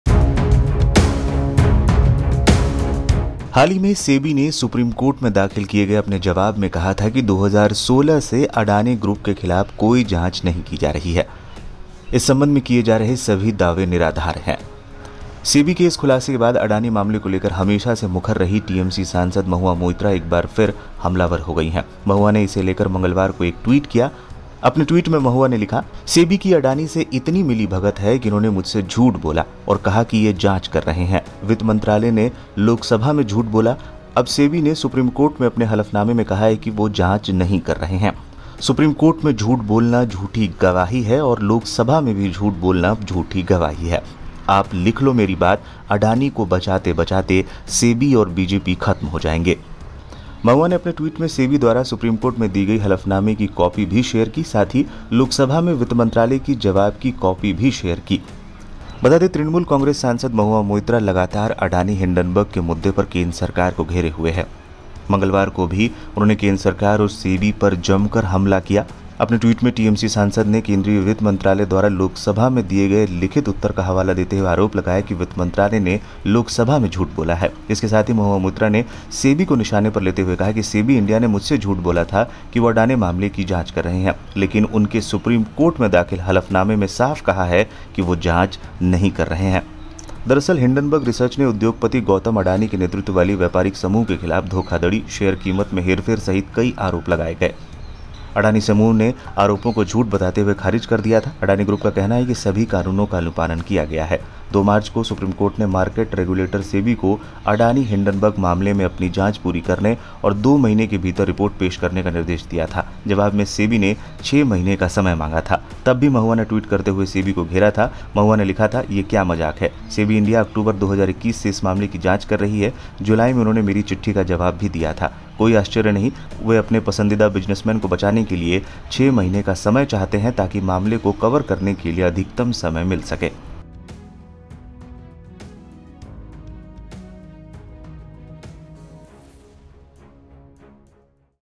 Mahua Moitra ने Adani-Hindenburg Issue मामले पर Nirmala Sitharaman और SEBI को घेर लिया
तृणमूल कांग्रेस सांसद महुआ मोइत्रा ने अडानी-हिंडनबर्ग के मुद्दे पर एक बार फिर केंद्र सरकार को निशाने पर लिया है,  महुआ मोइत्रा ने मंगलवार  को केंद्र सरकार और सेबी  पर हमला बोलते हुए एक ट्वीट किया। अपने ट्वीट में टीएमसी सांसद ने केंद्रीय वित्त मंत्रालय द्वारा लोक  सभा में दिए एक लिखित उत्तर का हवाला देते हुए आरोप लगाया कि वित्त मंत्रालय ने संसद में झूठ बोला है, इसके साथ ही  महुआ मोइत्रा सेबी को निशाने पर लेते हुए कहा सेबी इंडिया ने भी मुझसे झूठ बोला की वो अडानी मामले की जांच कर रहे हैं लेकिन उनके सुप्रीम कोर्ट में दाखिल हलफनामें में साफ कहा है कि वो जांच नहीं कर रहे है।महुआ ने अपने ट्वीट में सेबी और वित्त मंत्रालय को घेरते हुए ट्वीट किया,महुआ ने इस दौरान सेबी द्वारा सुप्रीम कोर्ट में दी गई हलफनामे की कॉपी में शेयर की साथ ही लोकसभा में वित्त मंत्रालय के जवाब की कॉपी भी शेयर की।